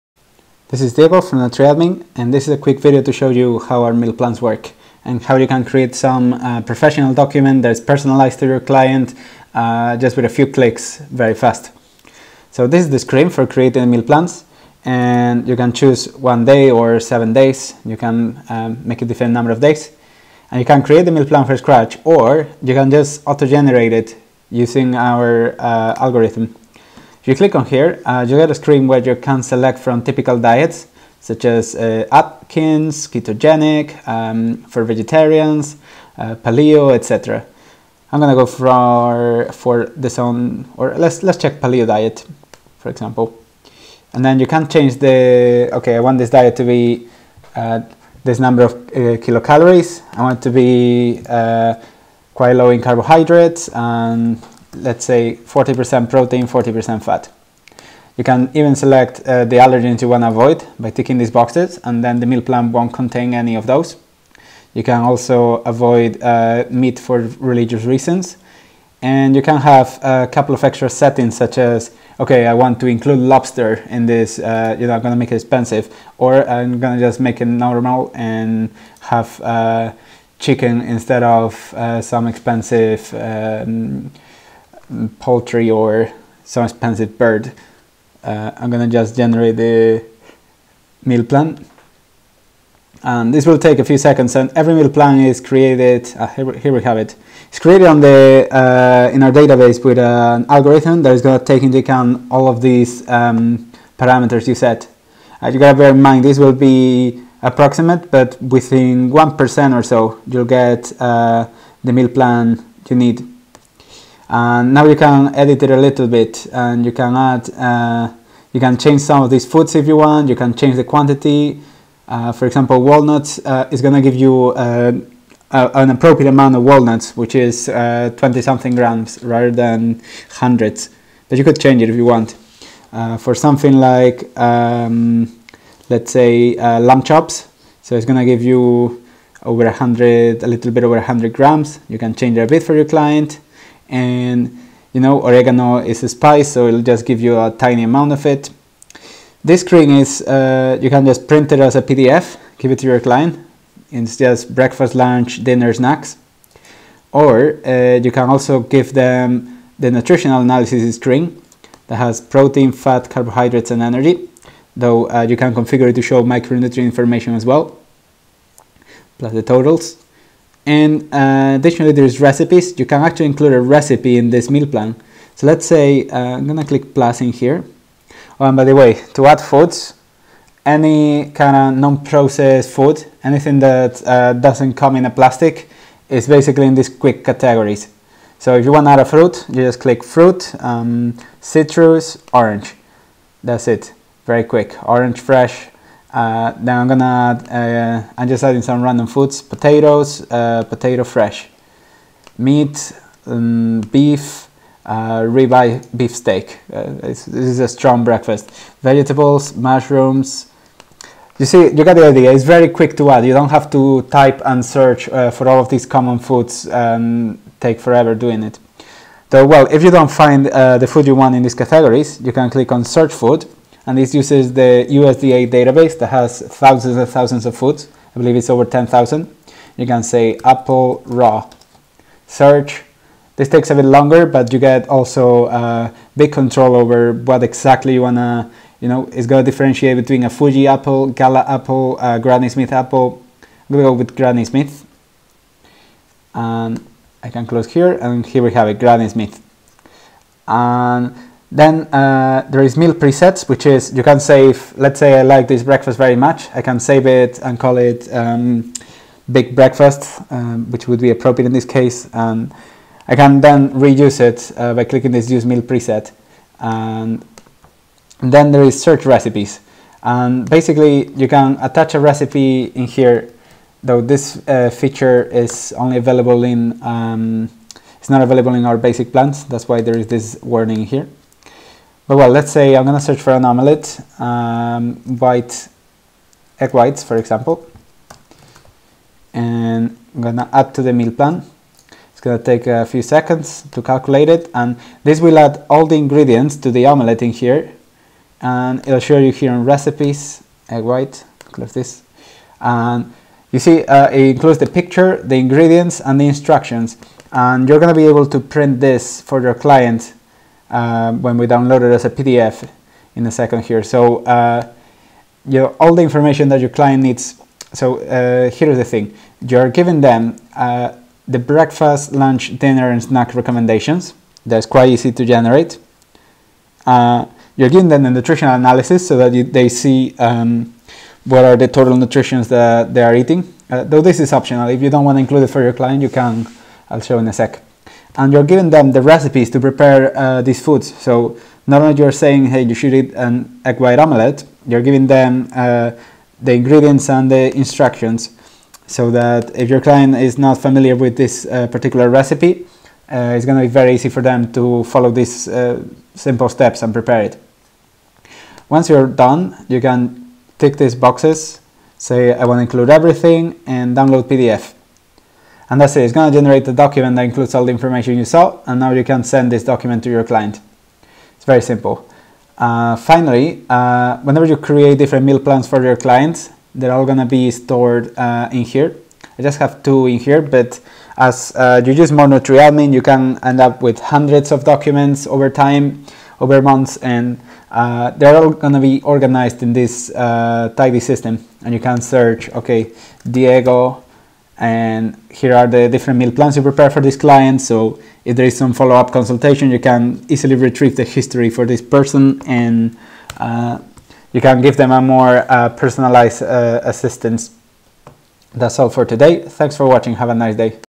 Meal Planning Software for Nutritionists | NutriAdmin
Generate professional meal plans for your clients with a few clicks in NutriAdmin. Choose macronutrient content, allergens to avoid, and select from popular diet plans to generate your plan. Include nutritional info effortlessly, and add recipes to the mix.